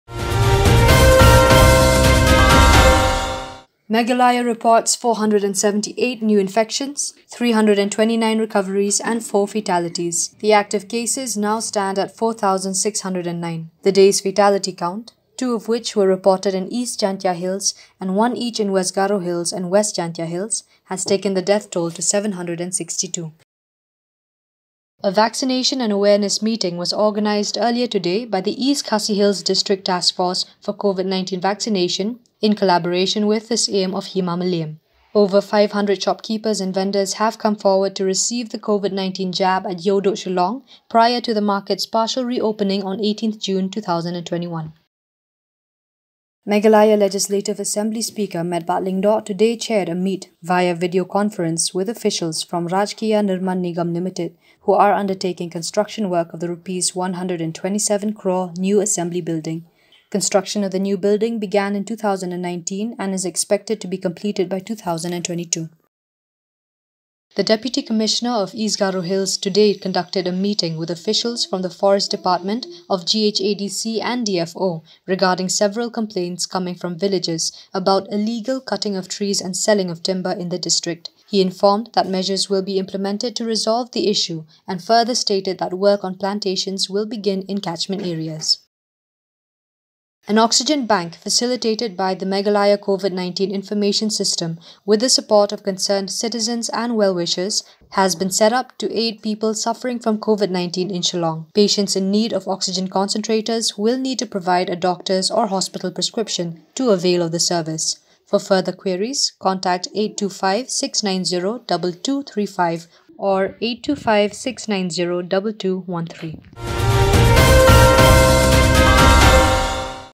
News Wrap Up | June 17, 2021
- Covid Update: 478 new infections, 329 recoveries, 4 deaths
- Over 500 people vaccinated in drive prior to reopening of Iewduh
- Review meeting held on new Assembly building construction
- Illegal tree cutting, timber sales prompts meeting with DC, East Garo Hills
- Oxygen aid services with O2 concentrators set up in Shillong